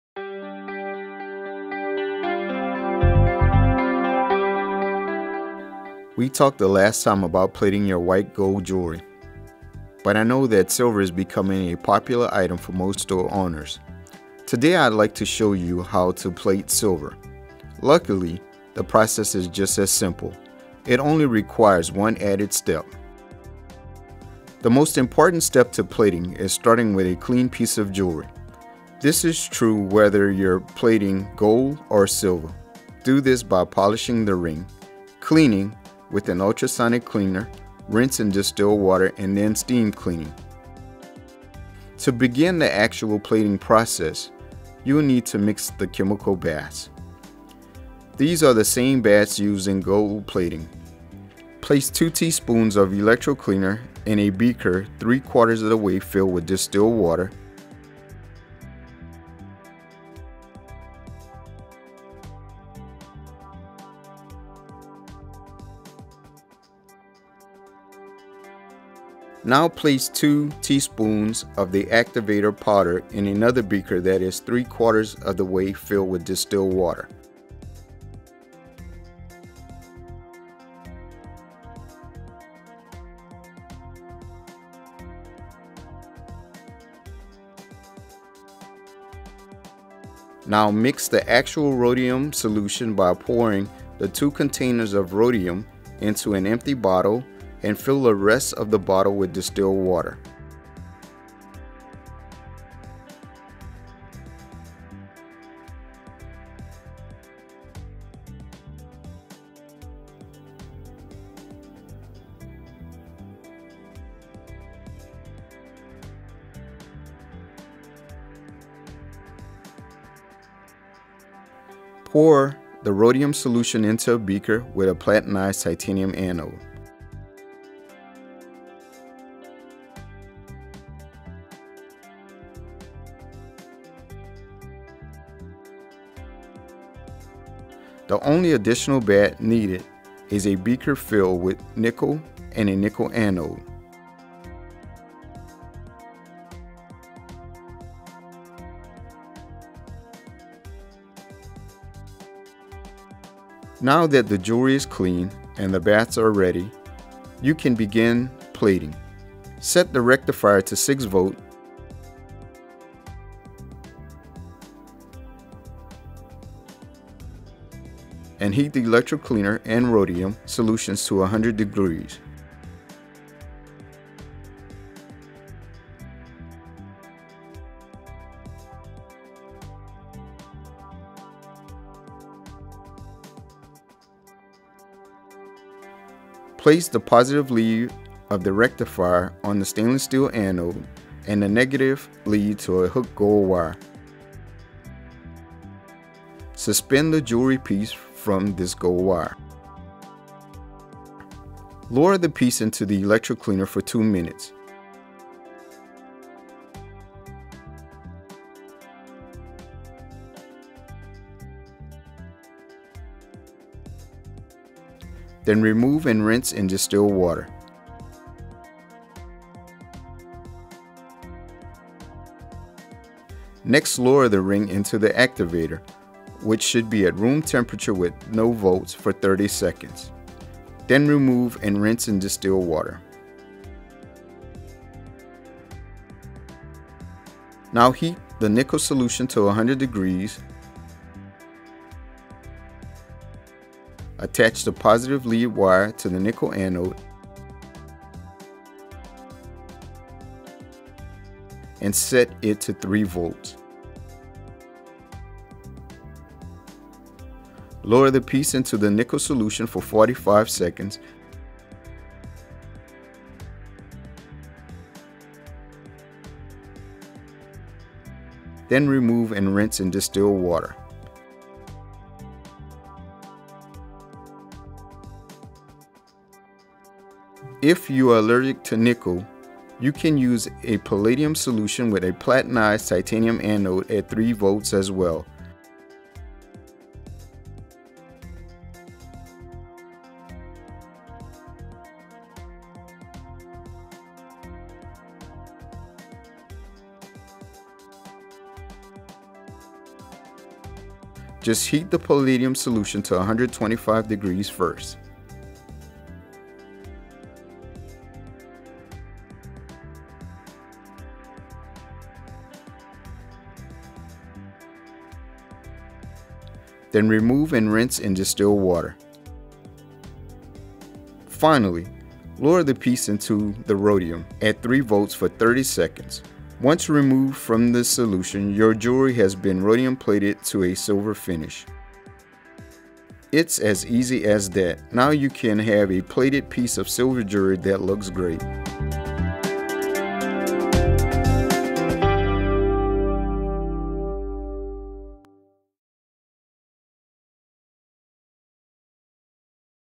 How to Rhodium Plate Sterling Silver Jewelry
The Stuller team walks through the process of rhodium plating silver.